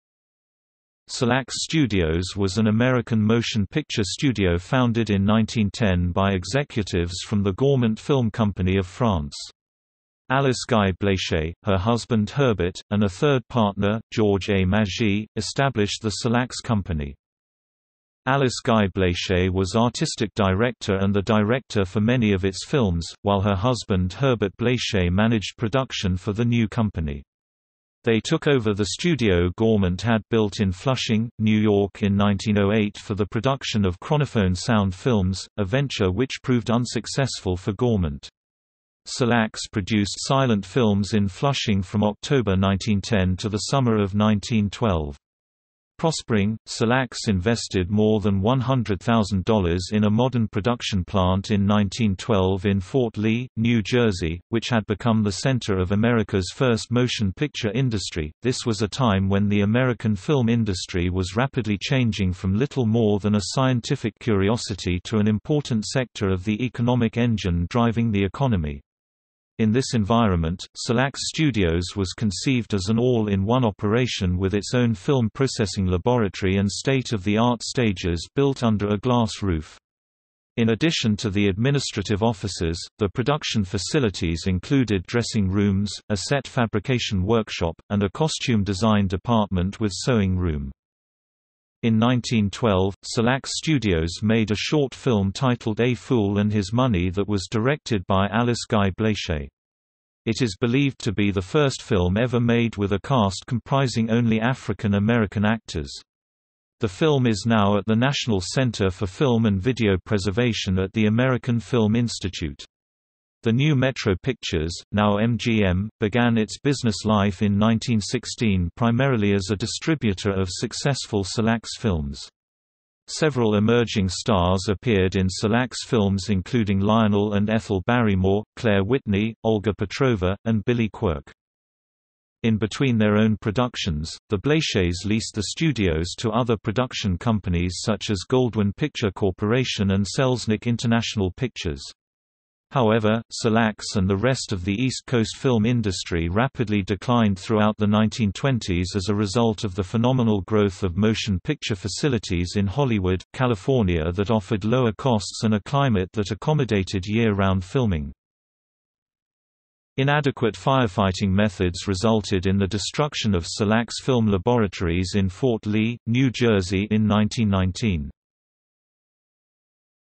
The Solax Company | Wikipedia audio article
- Socrates

SUMMARY
Solax Studios was an American motion picture studio founded in 1910 by executives from the Gaumont Film Company of France. Alice Guy-Blaché, her husband Herbert, and a third partner, George A. Magie, established the Solax Company.
Alice Guy-Blaché was artistic director and the director for many of its films, while her husband Herbert Blaché managed production for the new company. They took over the studio Gaumont had built in Flushing, New York in 1908 for the production of Chronophone sound films, a venture which proved unsuccessful for Gaumont. Solax produced silent films in Flushing from October 1910 to the summer of 1912. Prospering, Solax invested more than $100,000 in a modern production plant in 1912 in Fort Lee, New Jersey, which had become the center of America's first motion picture industry.This was a time when the American film industry was rapidly changing from little more than a scientific curiosity to an important sector of the economic engine driving the economy. In this environment, Solax studios was conceived as an all-in-one operation with its own film processing laboratory and state of the art stages built under a glass roof. In addition to the administrative offices, the production facilities included dressing rooms, a set fabrication workshop, and a costume design department with sewing room.
In 1912, Solax Studios made a short film titled A Fool And His Money that was directed by Alice Guy-Blaché. It is believed to be the first film ever made with a cast comprising only African-American actors. The film is now at the National Center for Film and Video Preservation at the American Film Institute. The new Metro Pictures, (now MGM), began its business life in 1916 primarily as a distributor of successful Solax films. Several emerging stars appeared in Solax films including Lionel and Ethel Barrymore, Claire Whitney, Olga Petrova, and Billy Quirk.
In between their own productions, the Blachés leased the studios to other production companies such as Goldwyn Picture Corporation and Selznick International Pictures. However, Solax and the rest of the East Coast film industry rapidly declined throughout the 1920s as a result of the phenomenal growth of motion picture facilities in Hollywood, California that offered lower costs and a climate that accommodated year-round filming.
Inadequate firefighting methods resulted in the destruction of Solax Film Laboratories in Fort Lee, New Jersey in 1919.